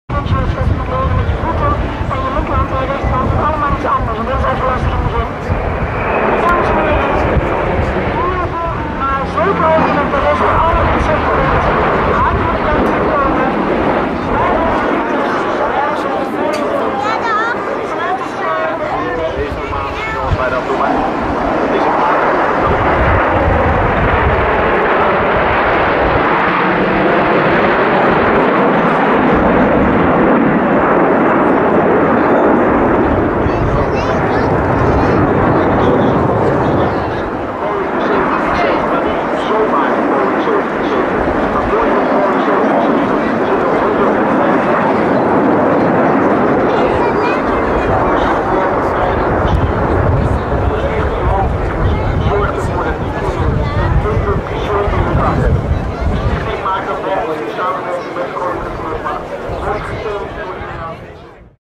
luchtmacht dagen 2013 Air Base Volkel: Flyby KLM Boeing 737 - 10x F-16 Formation
Flyby KLM Boeing 737 plus 10x F-16 Formation er achter zich aan.In het KLM-vliegtuig 125 zieke kinderen waren namens de Stichting Hoogvliegers ze kunnen worden een piloot voor een dag.